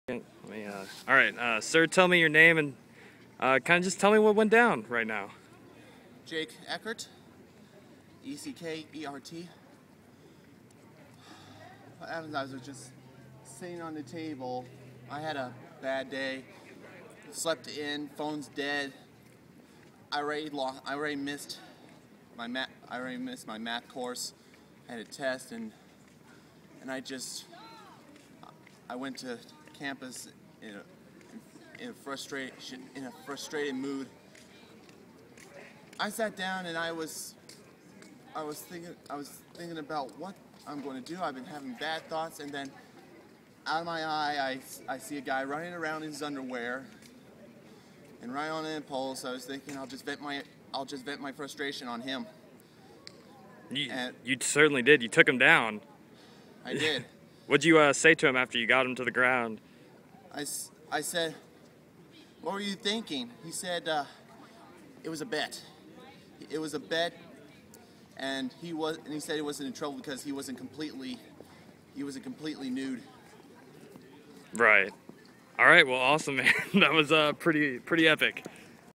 Interview with USIs Streaking Vigilante
A streakers romp across the quad was cut short by this random guy who apparently wasn't having it.